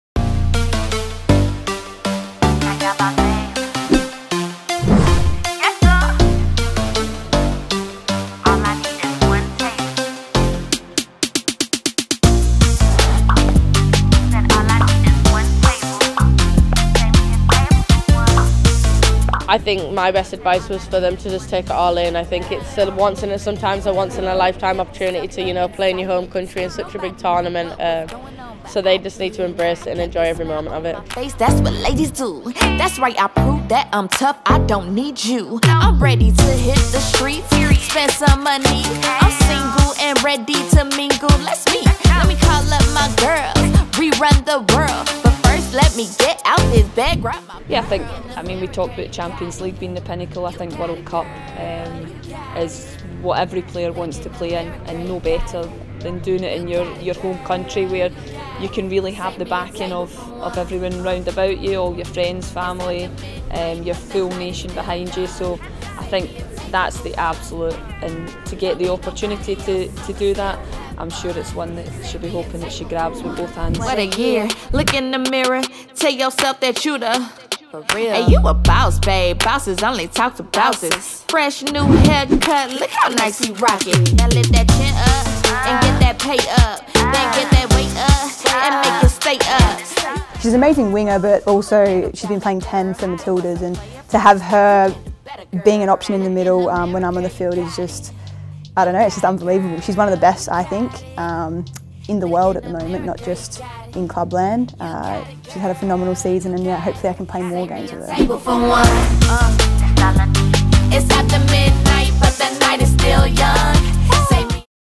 FIFA Women's World Cup Ones to Watch – Caitlin Foord
Fresh off a great campaign for Arsenal in the WSL Caitlin Foord will look to shine in her home World Cup for the Matildas.